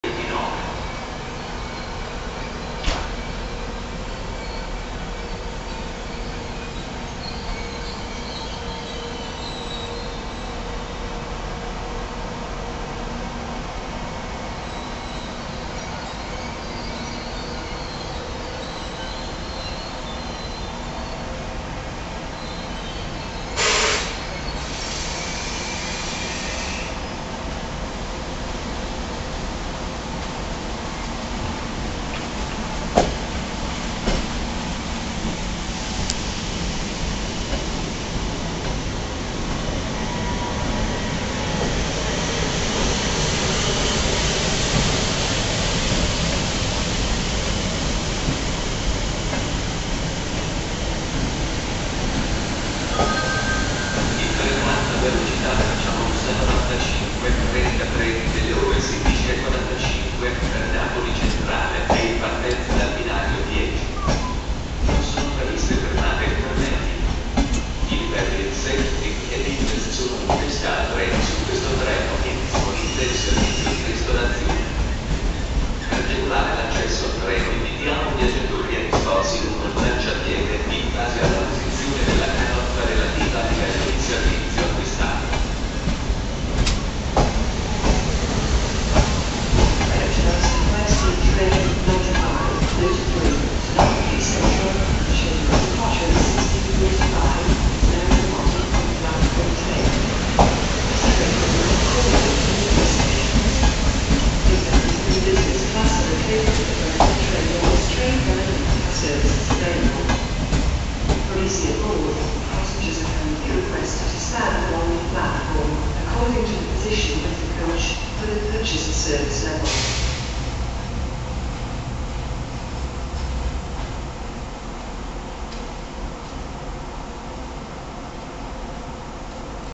ADDIO CARO E GLORIOSO PENDOLINO ETR 450!
Ecco a voi un triste evento che segnerà per sempre la storia delle Ferrovie dell Stato Italiane: la fine degli ETR 450. Ecco l'ultimo ETR 450 che parte oggi da Roma Termini: è il numero 02 che espleta il suo ultimo IC 561 Roma Termini - Reggio Calabria Centrale. La partenza, come potete vedere, è stata lenta e difficoltosa, quasi volesse farci capire che non ce la faceva più! Buona visione...